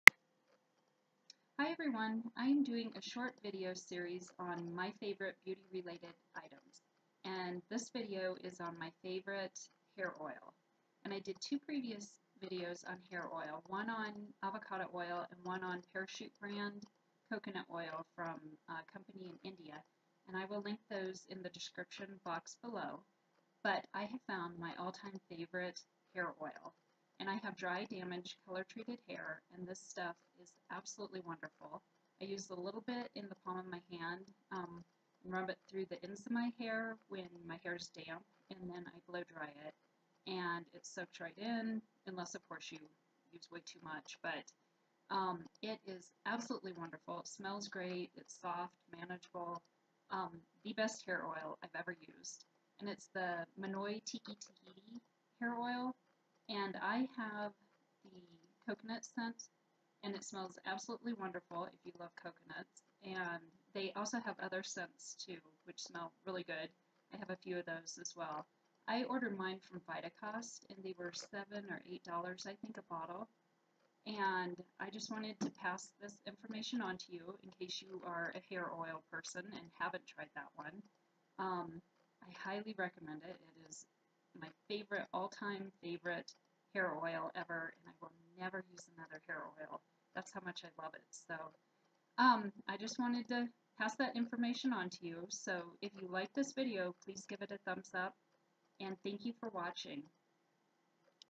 Monoi Tiki Tahiti Oil - My All Time Favorite Hair Oil
My All Time Favorites: Hair Oil: Monoi Tiki Tahiti Oil (Coco Coconut scent)
My other hair oil videos:
SHOP ALL MY FAVORITE PRODUCTS! Makeup, Body, Hair, Skincare, Microneedling & Fashion!
UNITED STATES
UNITED KINGDOM